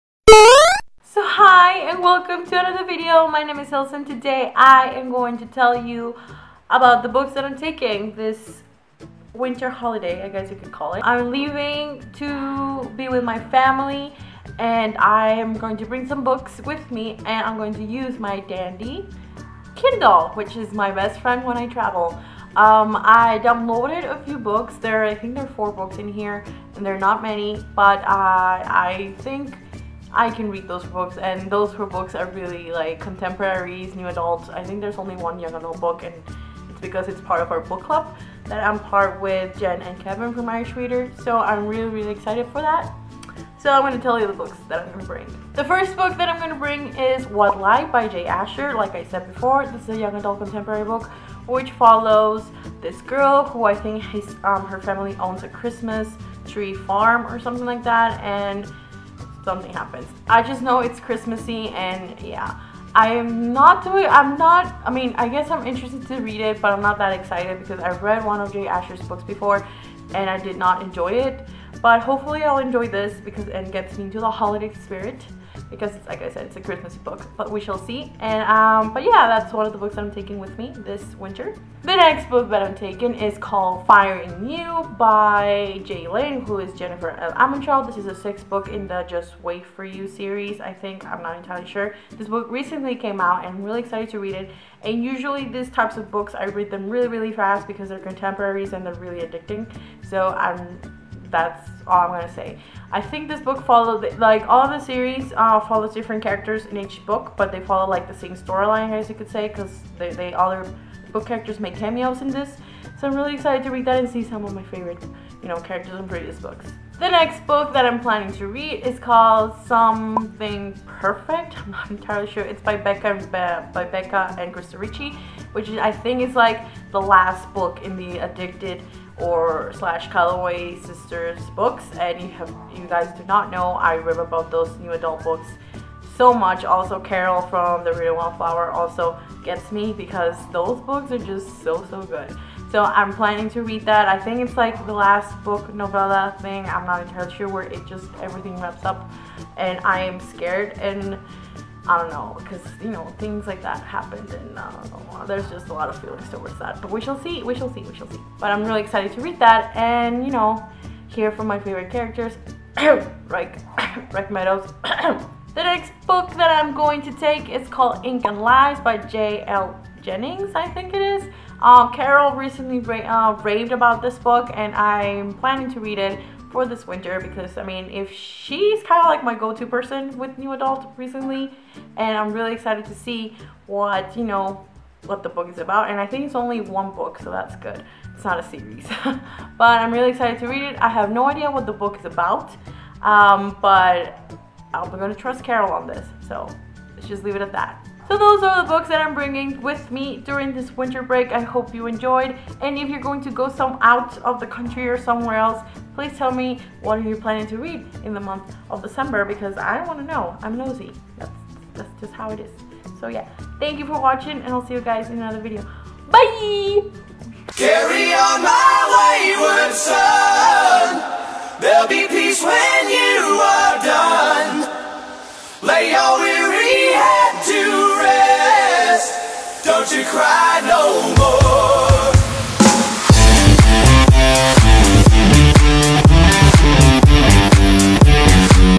DECEMBER TBR | 2016
Hi guys! this are the books that I took with me for my holiday!!
What are your December reads?!